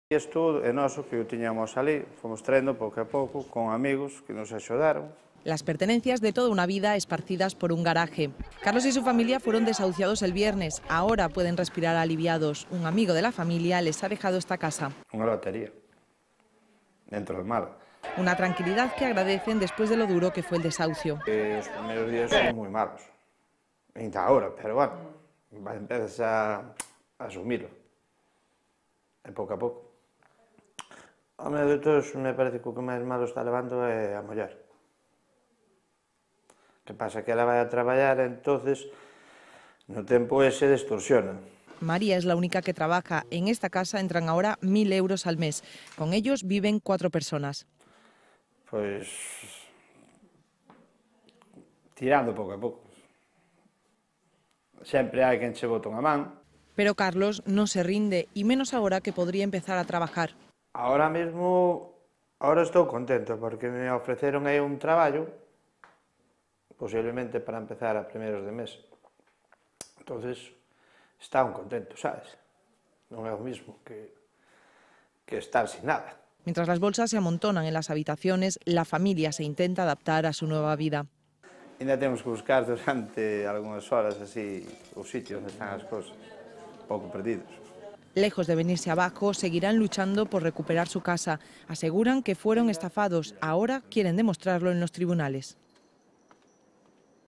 La vida tras el desahucio
La solidaridad ha hecho que la familia del barrio santiagués de Aríns desahuciada el pasado viernes, tenga un techo bajo el que dormir. Un amigo de los afectados les ha prestado gratuitamente una casa que no usaba. Poco a poco la familia intenta recuperar la normalidad